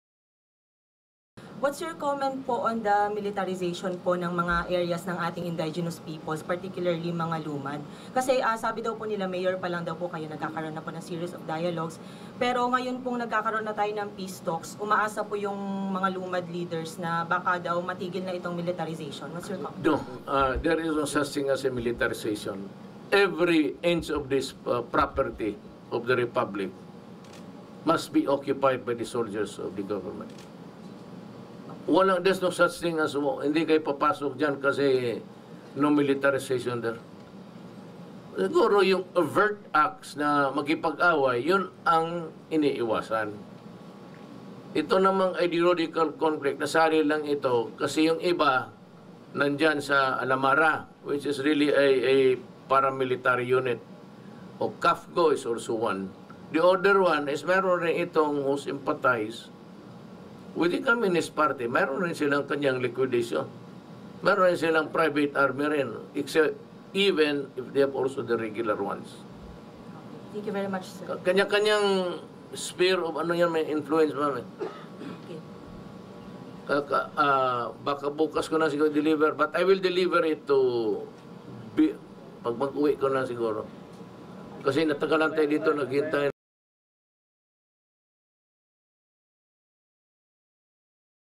President Duterte: Militarization does not exist in the country
Militarization does not exist in the country despite the deployment of troops to protect and defend the nation from security threats, President Duterte declared on Thursday.